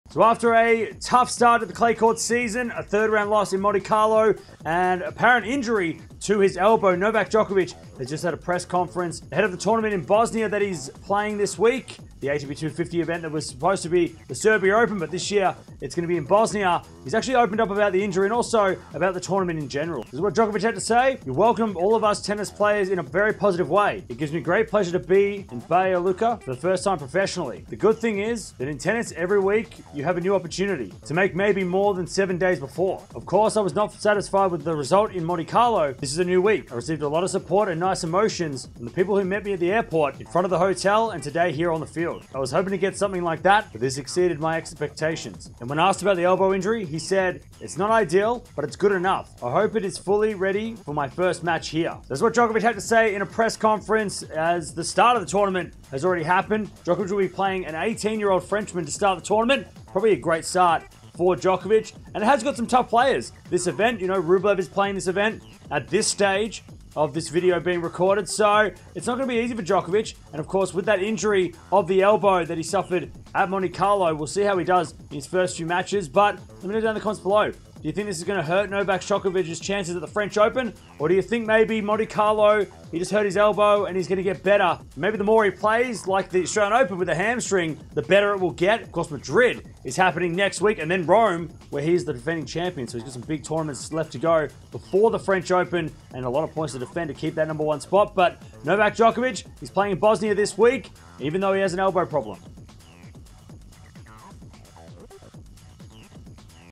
Djokovic Injury Update ahead of Madrid Open 2023 | Tennis Talk News
Novak Djokovic has confirmed his injury during the Monte Carlo Masters 2023 during a press conference at the Srpska Open 2023. Djokovic said that he hurt his elbow in Monte Carlo and he would see how it goes before his first match on Wednesday. Will he risk playing ahead of the bigger events in Madrid and Rome?

0:00 | Intro
0:21 | Press Conference
0:52 | Outro